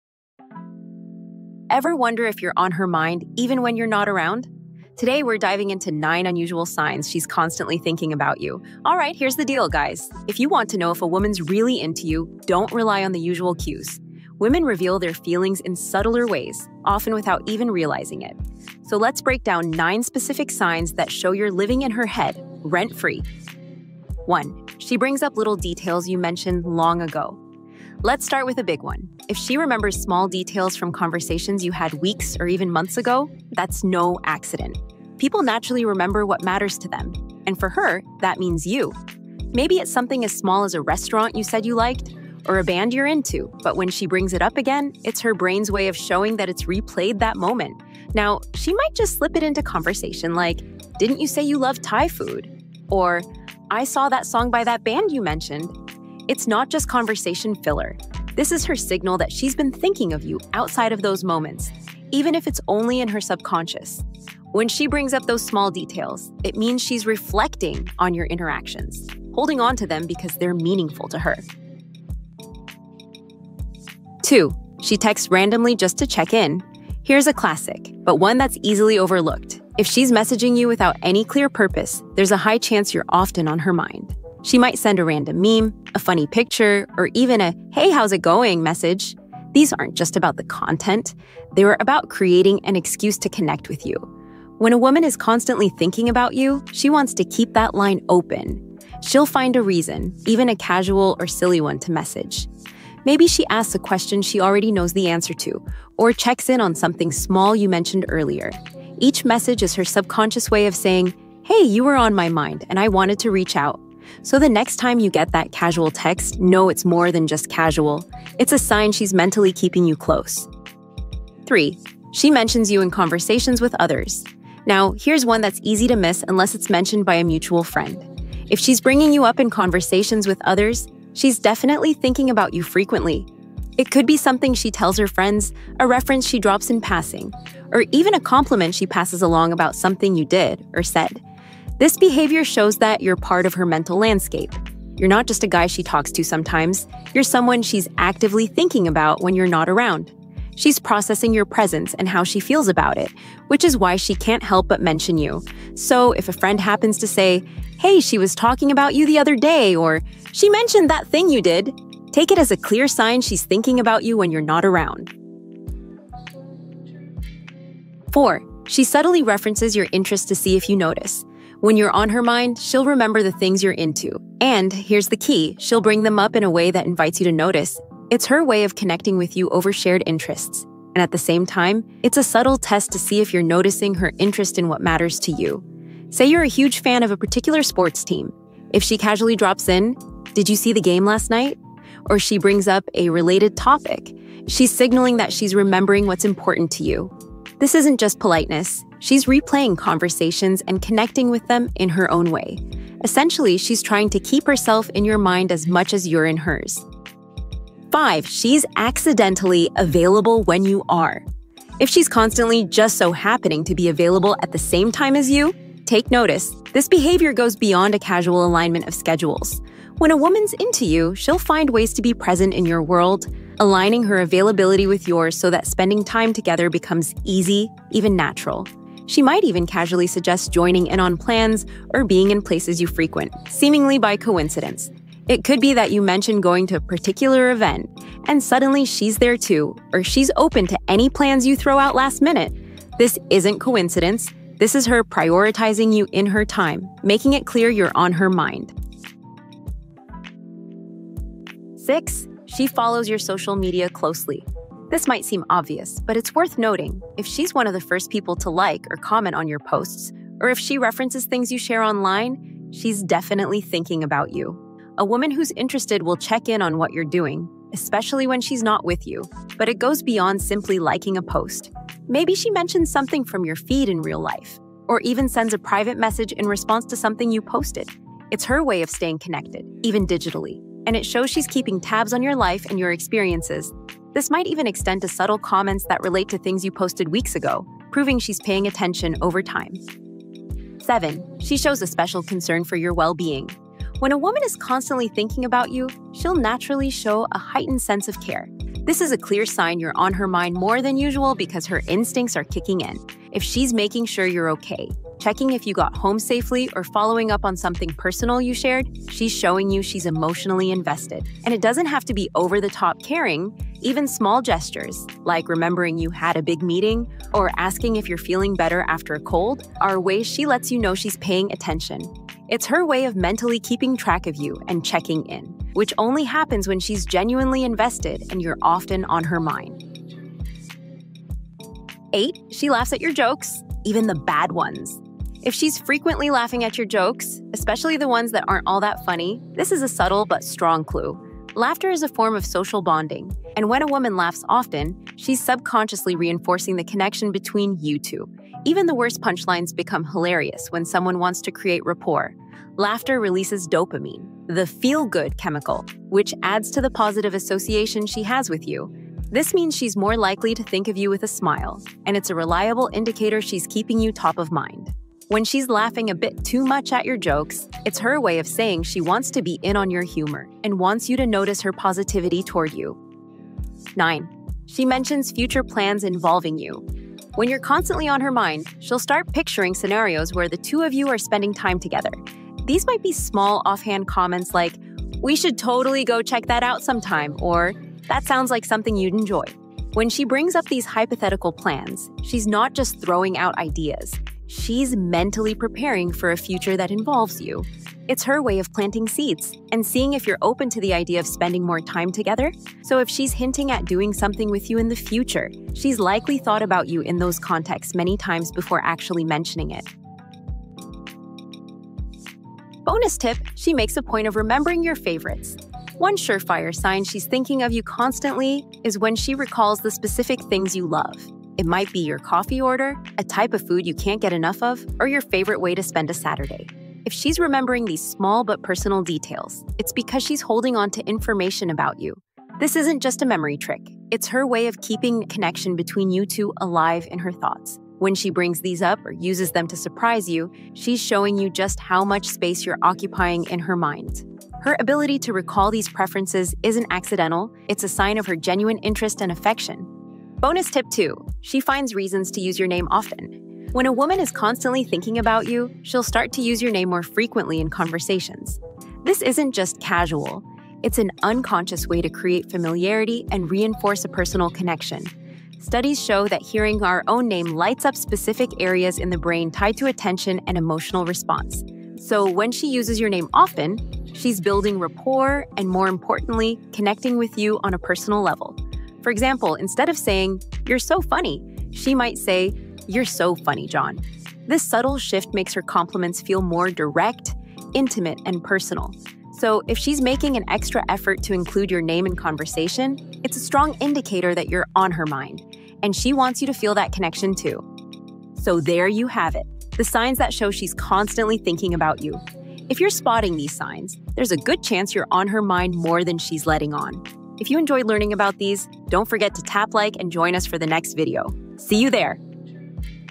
You're Constantly on Her Mind if You Notice THESE 9 Signs
Ever wonder if you’re on her mind even when you’re not around? Today, we’re diving into nine unusual signs she’s constantly thinking about you.

If you want to know if a woman’s really into you, don’t rely on the usual cues. Women reveal their feelings in subtler ways, often without even realizing it. So let’s break down nine specific signs that show you’re living in her head, rent-free.

Audio & Video Production by Afterschool Psychology